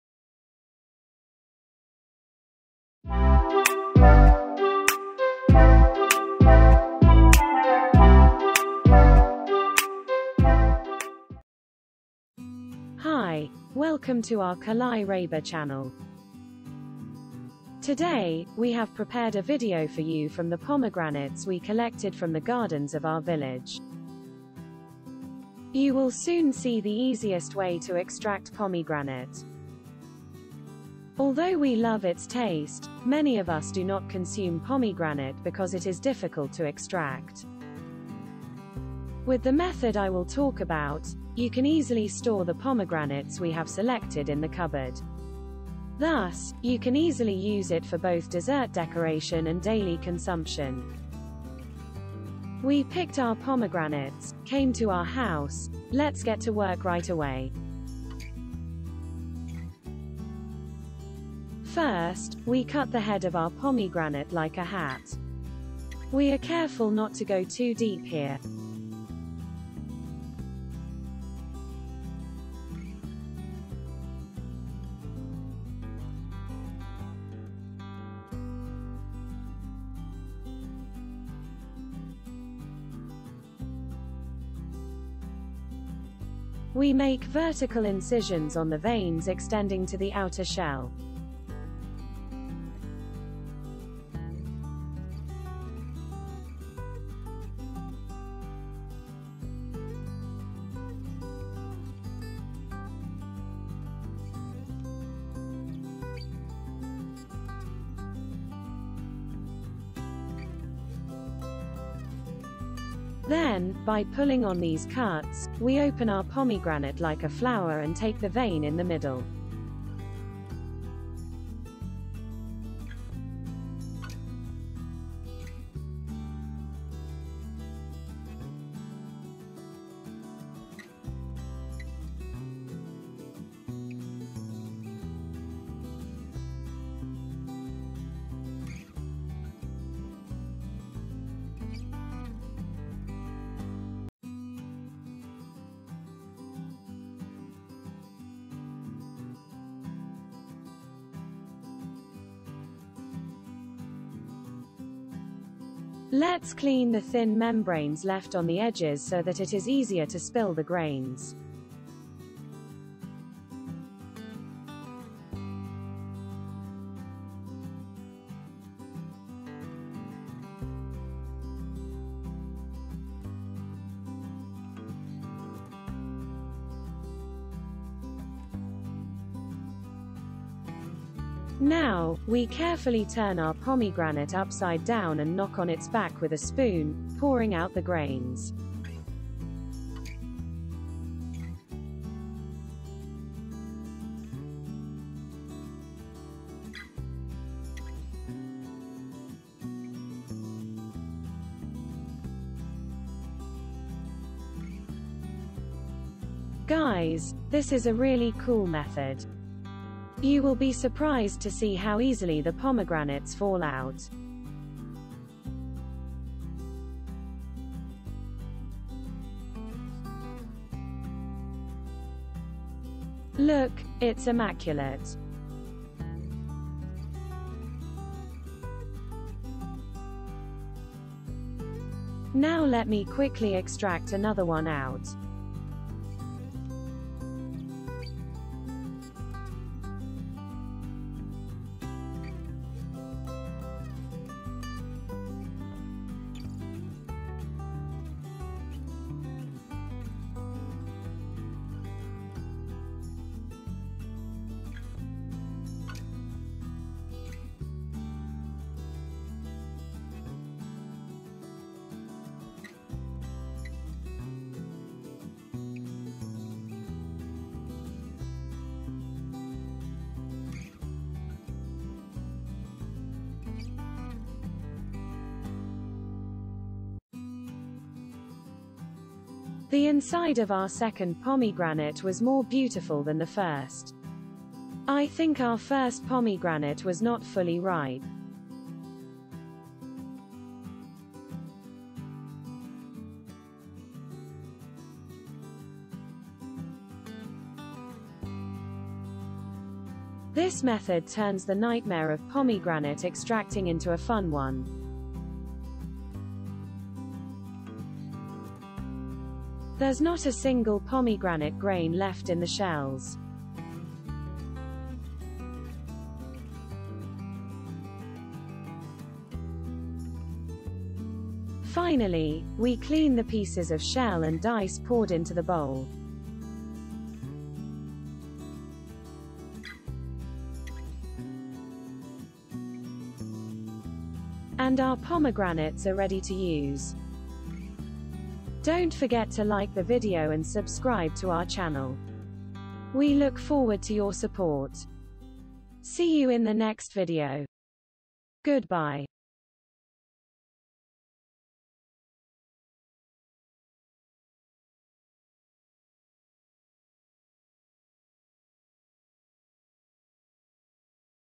AWESOME METHOD ! THE EASIEST METHOD OF PEELİNG POMEGRANATE ! PRACTICAL POMEGRANATE EXTRACTION !
AWESOME METHOD ! THE EASIEST METHOD OF PEELİNG POMEGRANATE ! PRACTICAL POMEGRANATE EXTRACTION ! Pomegranate lovers, we're getting rid of the problem of pomegranate extraction. I hear you who say that I love to eat pomegranates, but it is very difficult to peel. I suggest you watch our video where we explain the easiest method of peeling a pomegranate.

Social Media and Communication